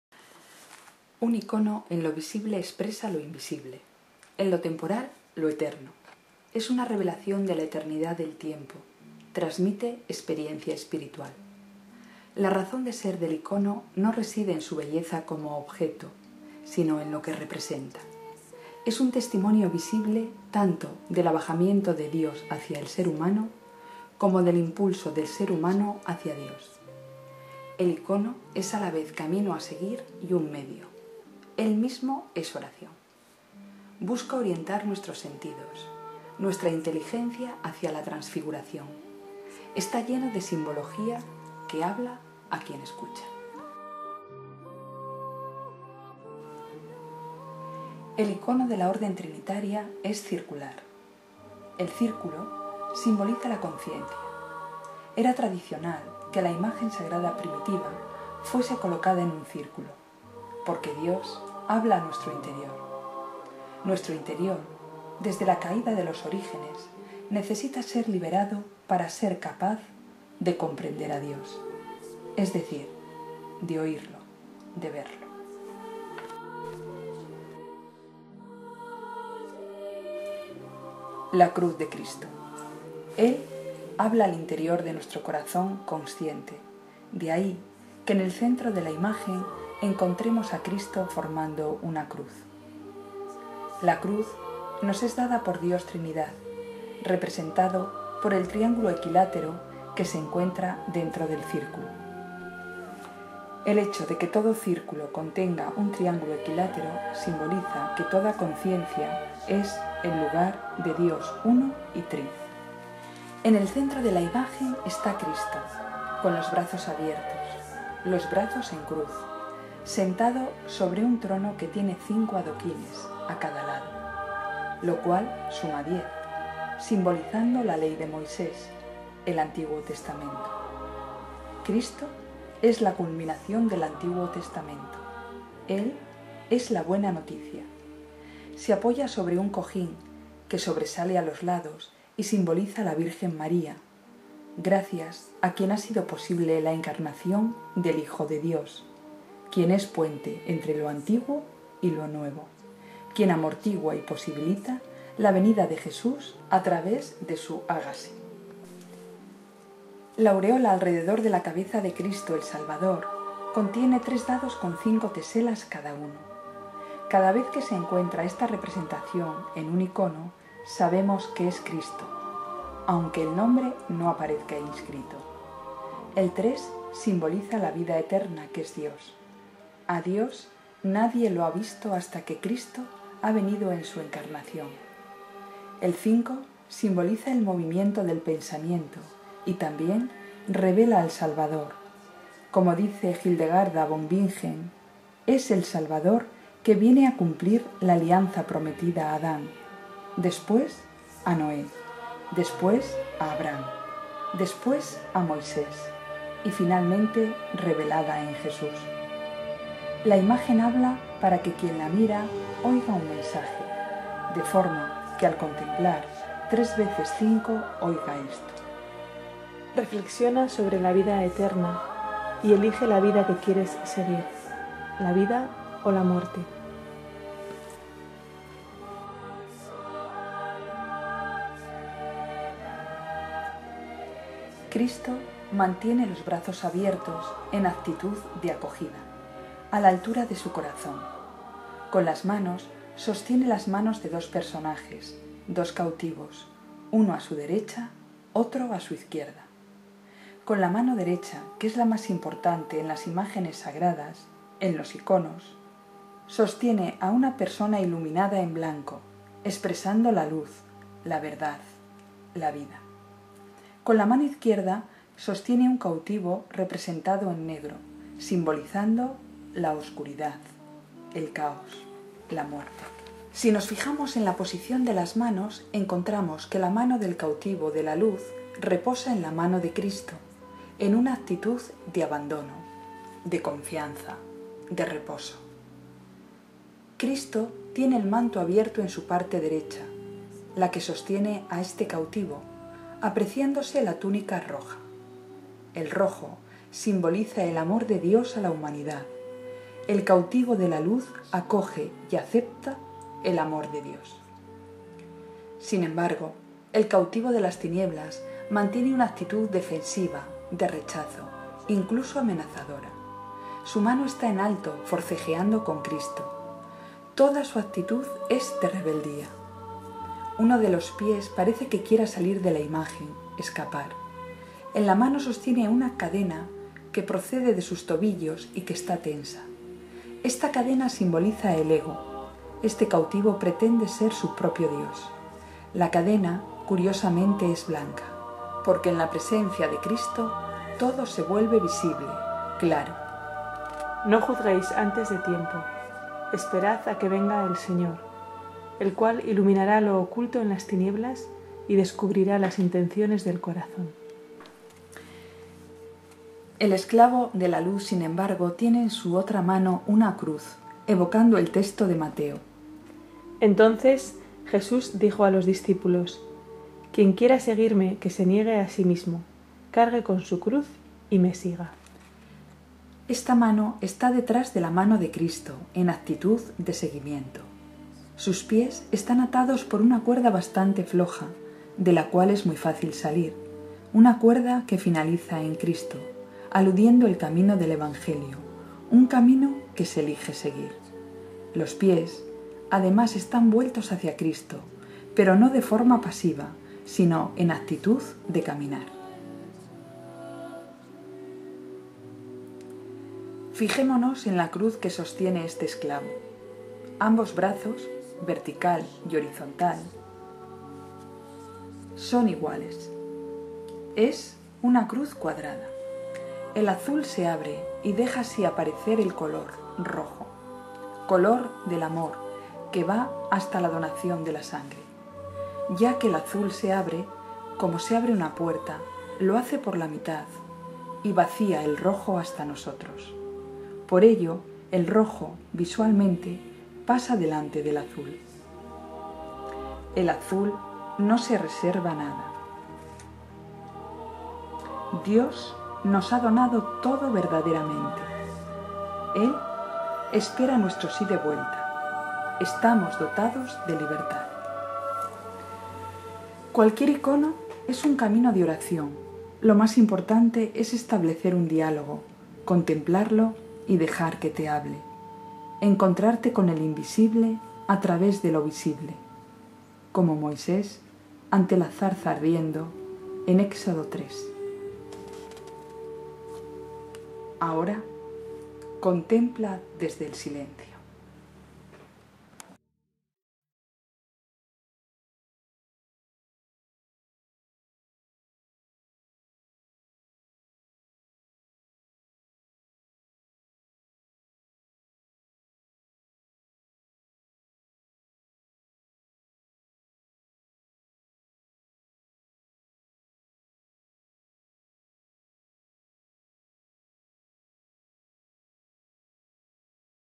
Icono de la orden de la Trinidad
Descripción del Icono de la Orden de la Santa Trinidad diseñado por San Juan de Mata, fundador de la Orden. Junto con la regla de vida, este icono es el legado que dejó a la Iglesia de su experiencia profunda de Dios.